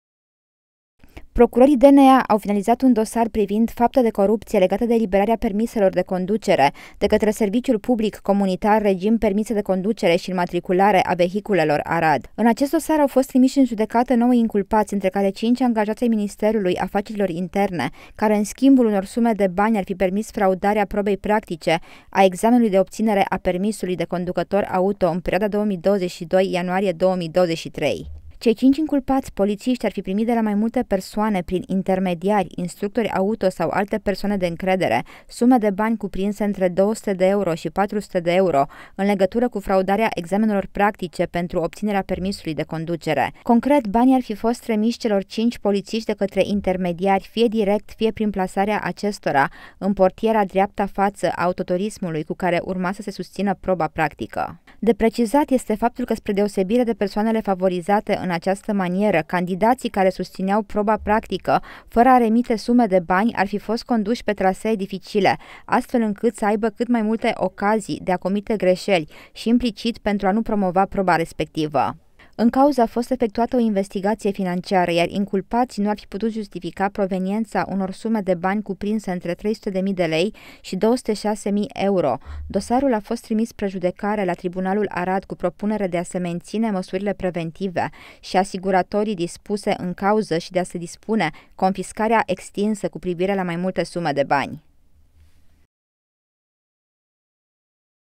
Mai mulți polițiști ar fi fraudat proba practică de obținere a permisului de conducere
Procurorii DNA au finalizat un dosar privind fapte de corupție legate de eliberarea permiselor de conducere de către Serviciului Public Comunitar Regim Permise de Conducere și Înmatriculare a Vehiculelor Arad. În acest dosar au fost trimiși în judecată nouă inculpați, între care cinci angajați ai Ministerului Afacerilor Interne care, în schimbul unor sume de bani, ar fi permis fraudarea probei practice a examenului de obținere a permisului de conducător auto în perioada 2022 – ianuarie 2023.